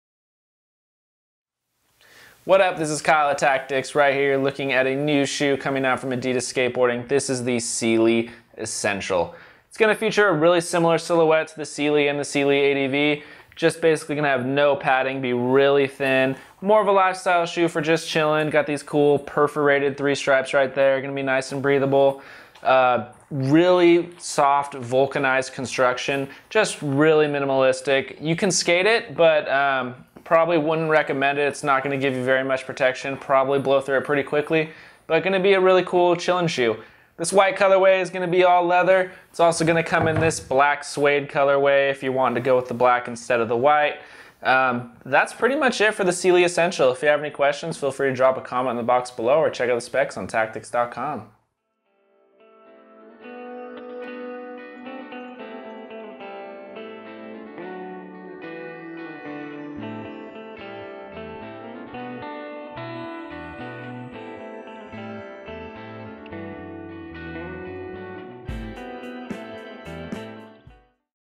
Adidas Seeley Essential Skate Shoes Review - Tactics
The Adidas Seeley Essential skate shoes are a cleaned up version of the original that you'll probably want to save for date nights. A deconstructed design makes these shoes the perfect chiller after a heavy session. Available in two colorways these things are all class and comfort.